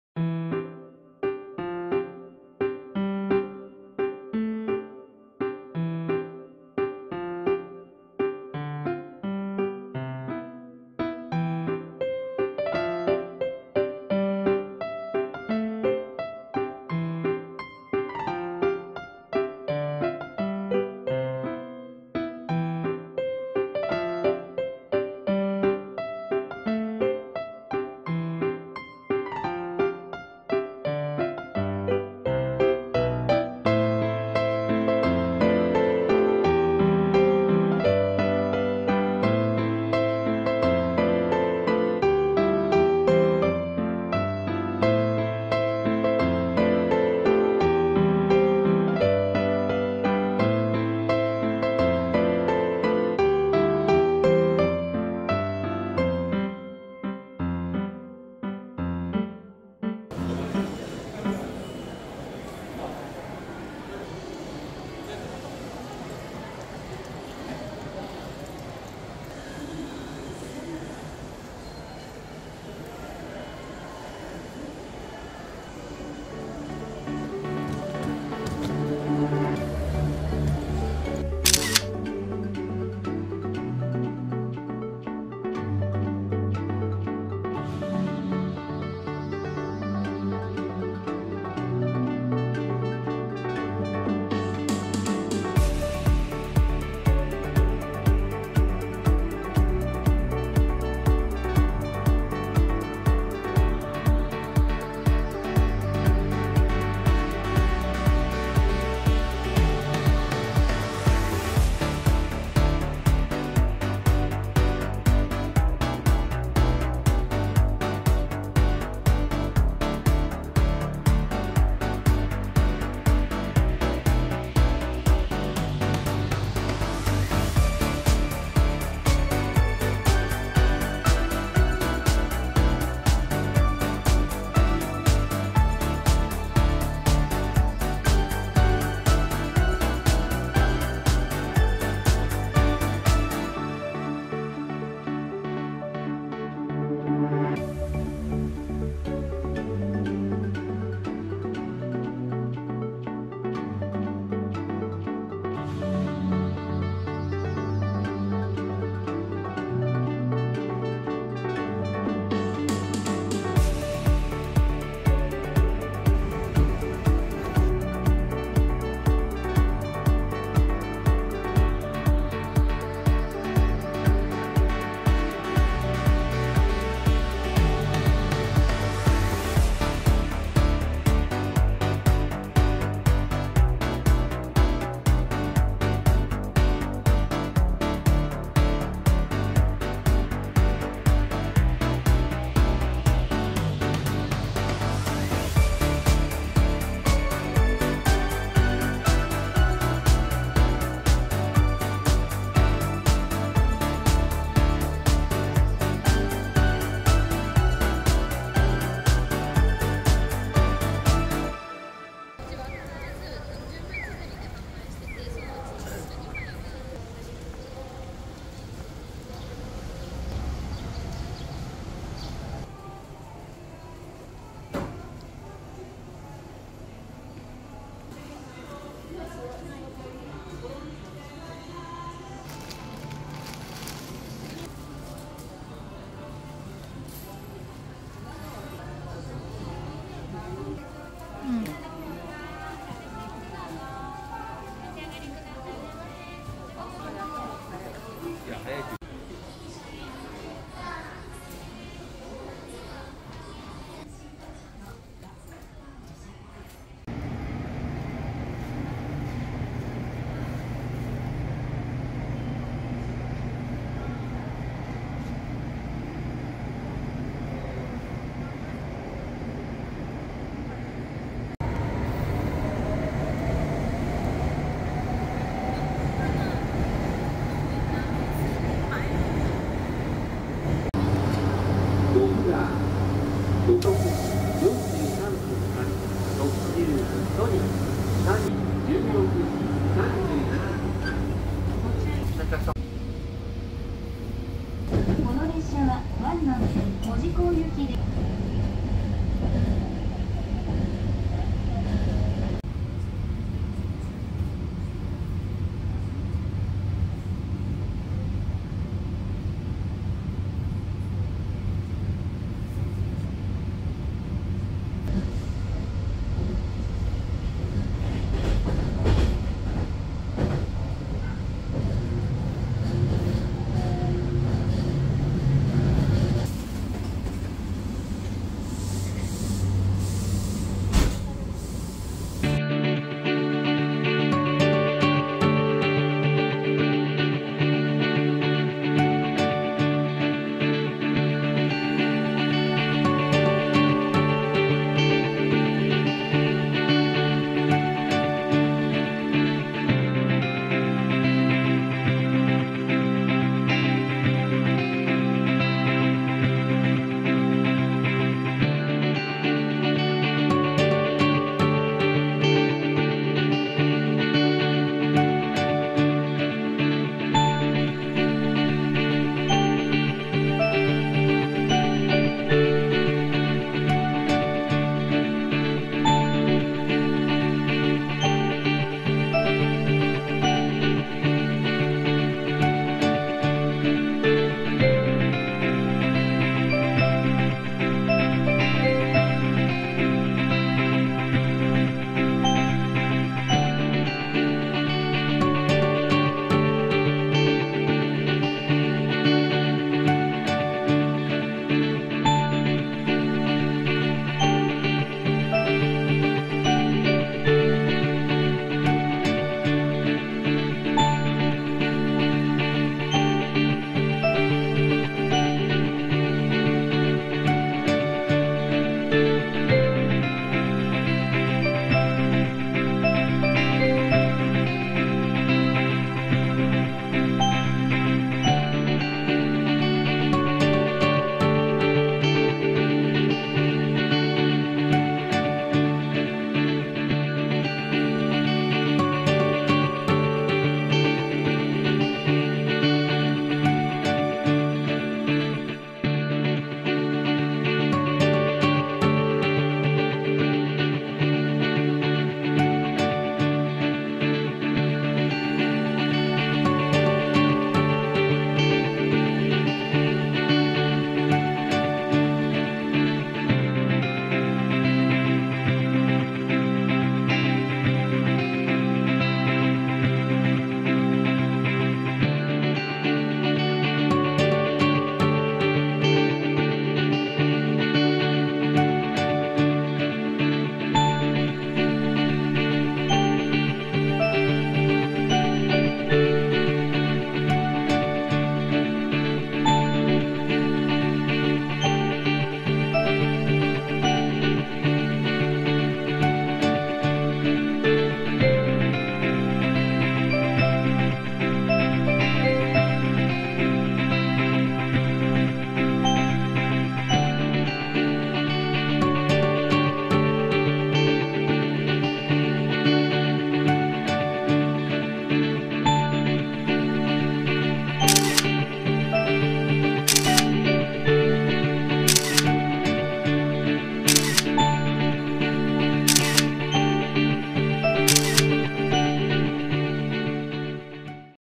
Kitakyushu Japan Vlog | Mojiko Retro | Day 1
Our family trip to Kitakyushu Japan! It has been such a long time since we went for a family trip~ We really enjoyed Kitakyushu and would like to share with everyone our experience!